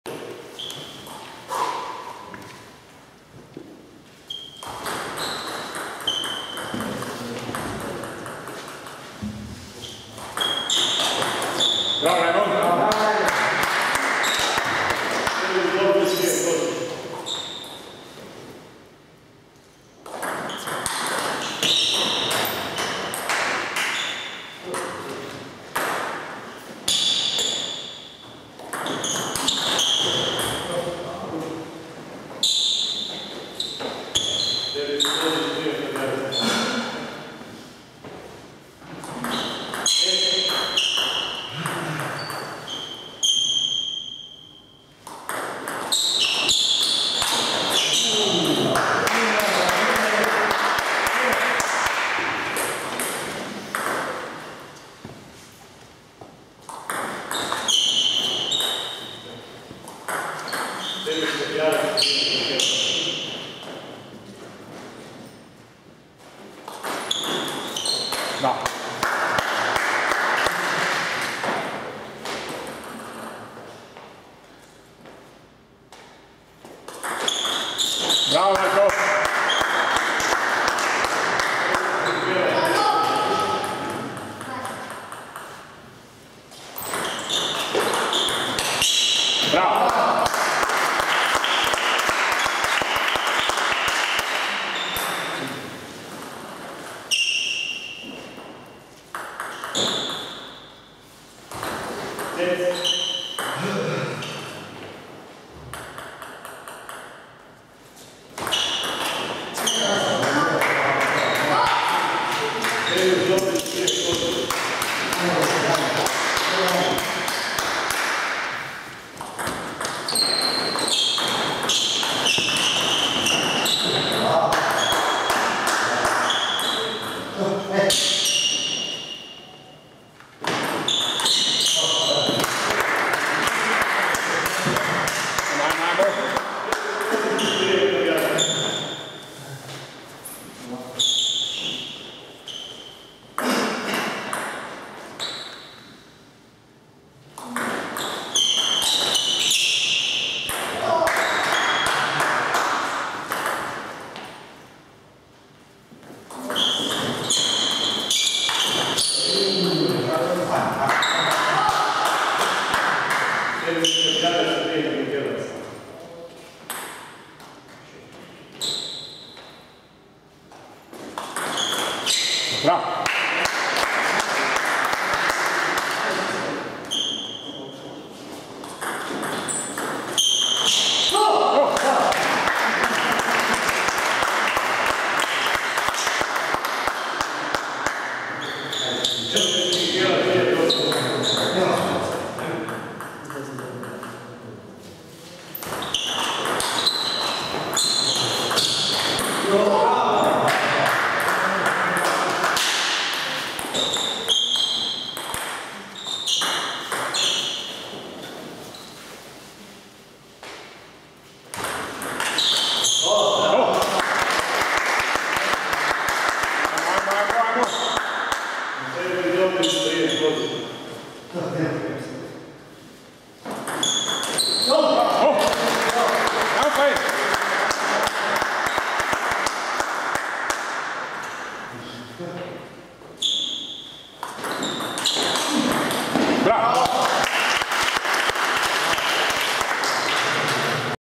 Habijanec, Dobrić - Kinkela, Kozić 3 : 0 (9, 9, 7), 1. set
7. kolo

Brovinje  - Opatija '08 4 : 1
Habijanec Marko - Salma Dominik 3 : 0 (3, 7, 5)
Dobrić Raymond - Kinkela Daniel 3 : 0 (13, 9, 11)
Jurišić Dario - Kozić Marijan 2 : 3 (5, 7, -8, -8, -8)
Habijanec, Dobrić - Kinkela, Kozić 3 : 0 (9, 9, 7)
Habijanec Marko - Kinkela Daniel 3 : 1 (-9, 5, 7, 9)